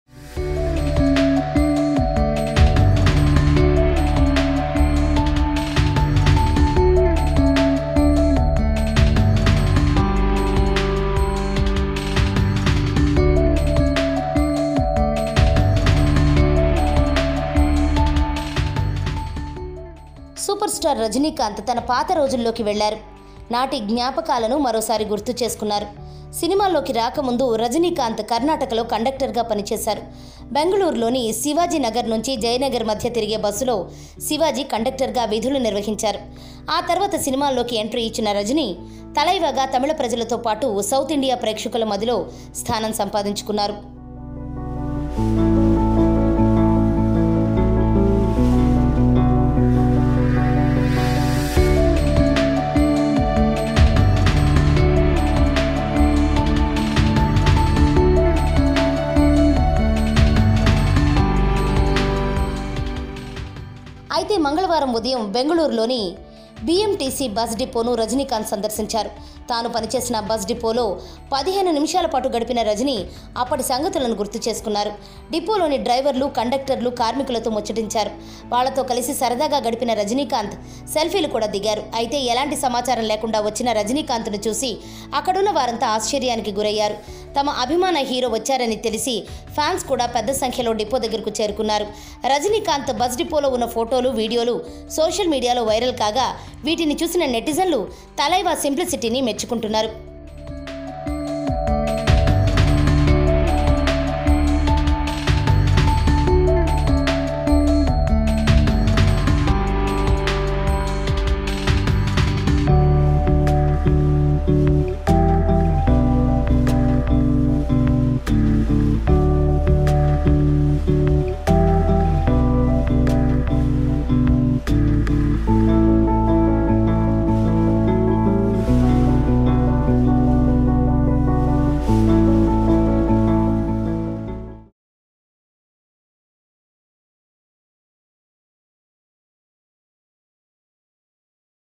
Superstar Rajnikanth Surprise Visit @BMTC Bus Depot In Bengaluru | Samayam Telugu
Superstar Rajnikanth Surprise Visit @BMTC Bus Depot In Bengaluru.